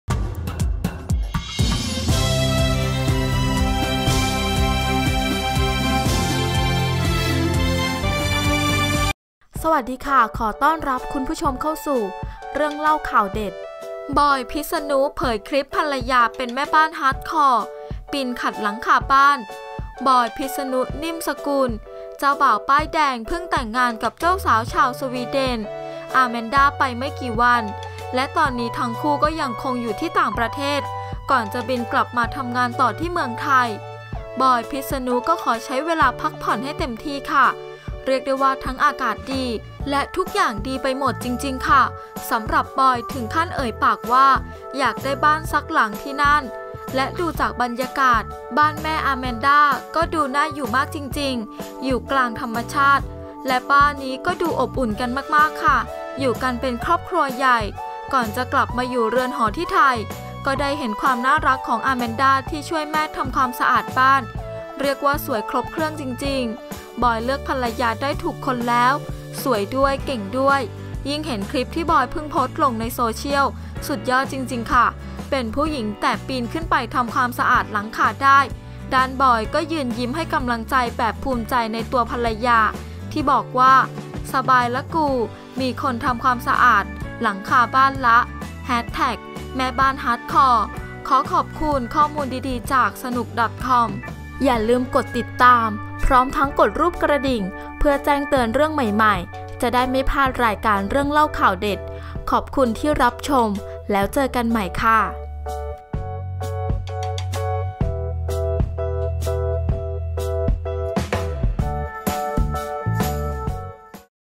"บอย พิษณุ" เผยคลิปภรรยาเป็นแม่บ้านฮาร์ดคอร์ ปีนขัดหลังคาบ้าน
บอย พิษณุ นิ่มสกุล เจ้าบ่าวป้ายแดง เพิ่งแต่งงานกับเจ้าสาวชาวสวีเดน อาแมนด้า ไปไม่กี่วัน และตอนนี้ทั้งคู่ก็ยังคงอยู่ที่ต่างประเทศ ก่อนจะบินกลับมาทำงานต่อที่เมืองไทย
บอย พิษณุ ก็ขอใช้เวลาพักผ่อนให้เต็มที่
เรียกว่าทั้งอากาศดีและทุกอย่างดีไปหมดจริงๆ สำหรับบอยถึงขั้นเอ่ยปากว่า อยากได้บ้านสักหลังที่นั่น และดูจากบรรยากาศ บ้านแม่อาแมนด้าก็ดูน่าอยู่มากจริงๆ อยู่กลางธรรมชาติ
และบ้านนี้ก็ดูอบอุ่นกันมากๆ อยู่กันเป็นครอบครัวใหญ่
ก่อนจะกลับมาอยู่เรือนหอที่ไทย ก็ได้เห็นความน่ารักของอาแมนด้าที่ช่วยแม่ทำความสะอาดบ้าน เรียกว่าสวยครบเครื่องจริงๆ บอยเลือกภรรยาได้ถูกคนแล้ว สวยด้วยเก่งด้วย
ยิ่งเห็นคลิปที่บอยเพิ่งโพสต์ลงโซเชียล สุดยอดจริงๆ เป็นผู้หญิงแต่ปีนไปทำความสะอาดหลังคาได้ ด้านบอยก็ยืนยิ้มให้กำลังใจแบบภูมิใจในตัวภรรยา ที่บอกว่า
"สบายละกรูมีคนทำความสะอาดหลังคาบ้านละ555 แม่บ้านฮาร์ดคอร์"